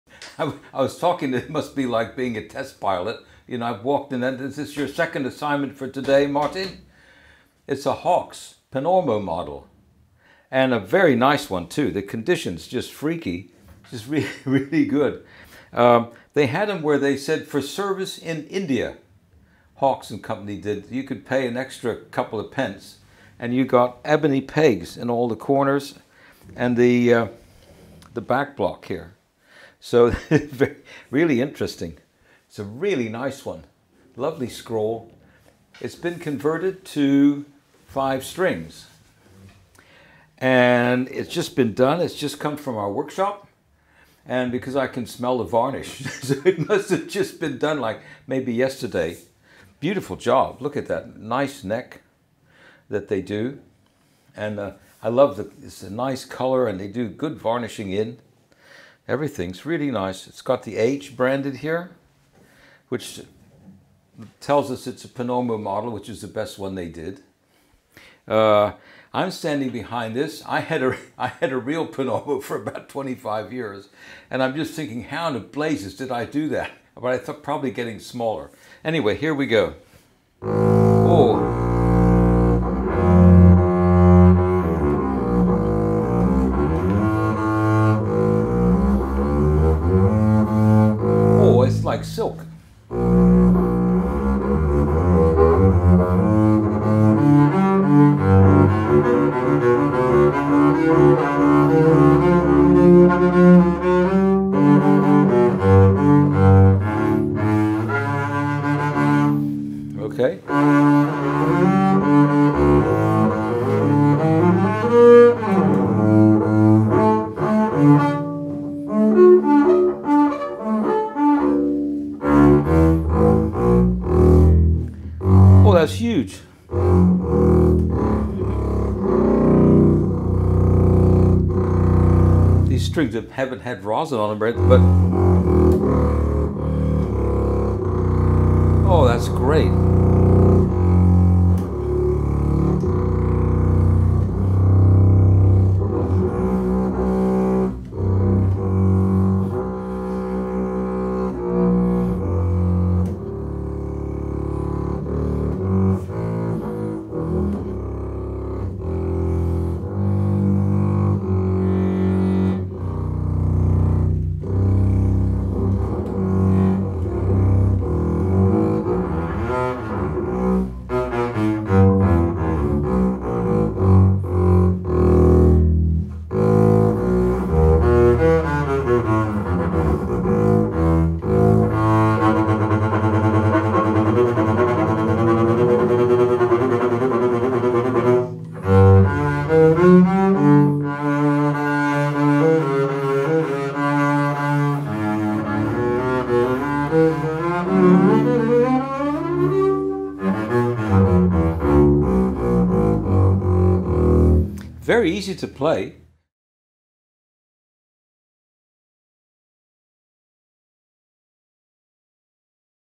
*SOLD* Hawkes & Son Panormo Model Double Bass: Played by Tom *SOLD*
*SOLD*  This mighty Hawkes & Son Panormo double bass is fresh from our workshop, having undergone a thorough restoration and conversion to five strings. Tom takes it for a spin, showing off its incredibly beefy sound in the lower registers.